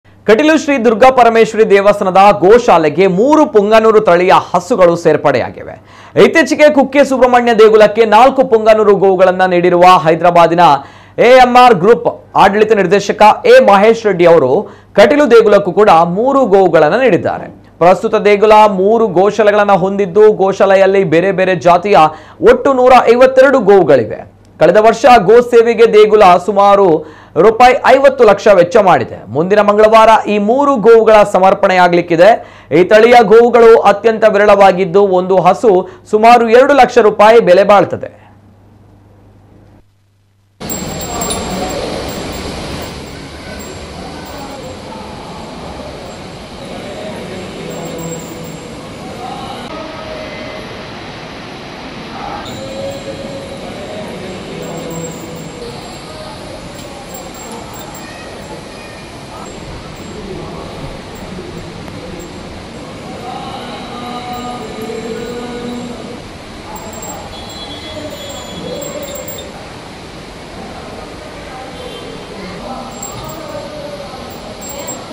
ಕಟೀಲು ನಂದಿನಿ ಮಡಿಲಿಗೆ ಲಕ್ಷಾಂತರ ರೂ ಮೌಲ್ಯದ ಪುಂಗನೂರು ತಳಿಯ ಗೋವು, ಉದ್ಯಮಿ ಮಹೇಶ್ ರೆಡ್ಡಿಯಿಂದ ಸಮರ್ಪಣೆ..!
nammakudlanews24x7 ಕಟೀಲು ಶ್ರೀ ದುರ್ಗಾಪರಮೆಶ್ವರೀ ದೇವಸ್ಥಾನದ ಗೋಶಾಲೆಗೆ ಮೂರು ಪುಂಗನೂರು ತಳಿಯ ಹಸುಗಳು ಸೇರ್ಪಡೆಯಾಗಿವೆ, ಇತ್ತೀಚಿಗೆ ಕುಕ್ಕೆ ಸುಬ್ರಹ್ಮಣ್ಯ ದೇಗುಲಕ್ಕೆ ನಾಲ್ಕು ಪುಂಗನೂರು ಗೋವುಗಳನ್ನು ನೀಡಿರುವ ಹೈದರಾಬಾದಿನ ಎ.ಎಂ.ಆರ್ ಗ್ರೂಪ್‌ನ ಆಡಳಿತ ನಿರ್ದೇಶಕ ಎ. ಮಹೇಶ್ ರೆಡ್ಡಿ ಅವರು ಕಟೀಲು ದೇಗುಲಕ್ಕೂ ಮೂರು ಗೋವುಗಳನ್ನು ನೀಡಿದ್ದಾರೆ.
ತಾಜಾ ಸುದ್ದಿಗಳಿಗೆ  ನಮ್ಮ ಕುಡ್ಲ whatapp ಗ್ರೂಪ್‌ಗೆ  ಸೇರಿ..